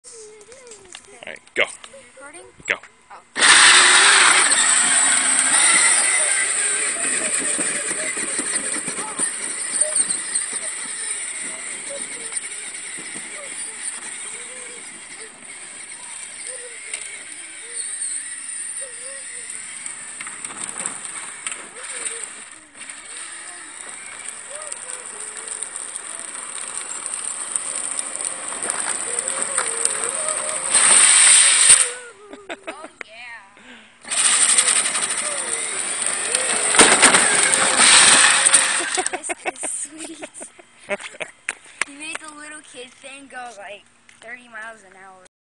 24v Power Wheels - Muddin' in Texas
Modified Power Wheels 4wheeler.